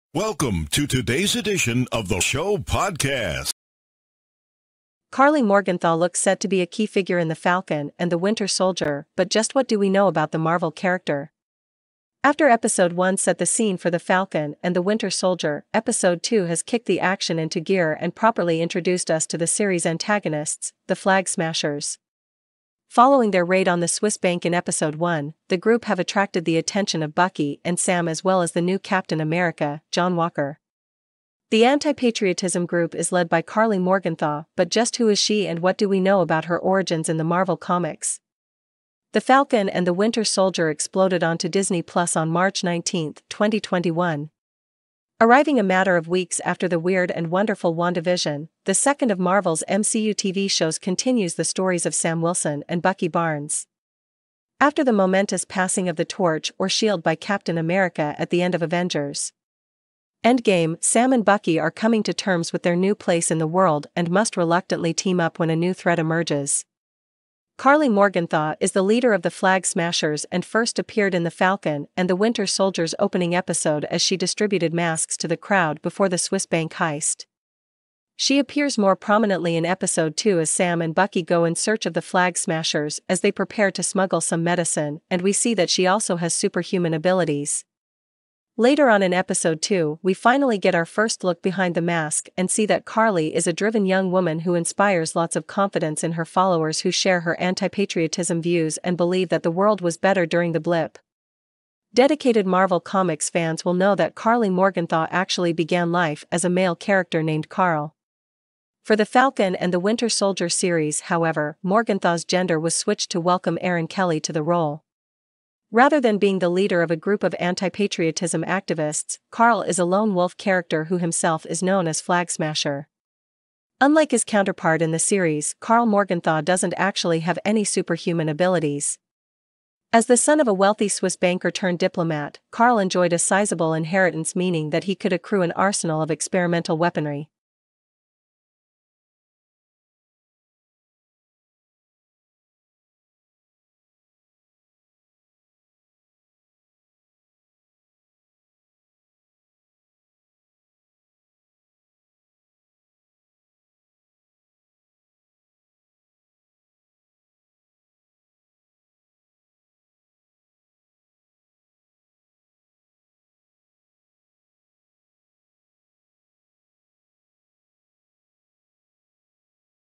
WHO IS KARLI MORGENTHAU? MARVEL’S FLAG-SMASHER IS KEY TO FALCON AND THE WINTER SOLDIER
Karli Morgenthau looks set to be a key figure in The Falcon and the Winter Soldier but just what do we know about the Marvel character?. . . . After episode 1 set the scene for The Falcon and the Winter Soldier, episode 2 has kicked the action into gear and properly introduced us to the series’ antagonists, the Flag Smashers.. . . . Following their raid on the Swiss bank in episode 1, the group have attracted the attention of Bucky and Sam as well as the new Captain America, John Walker.. . . .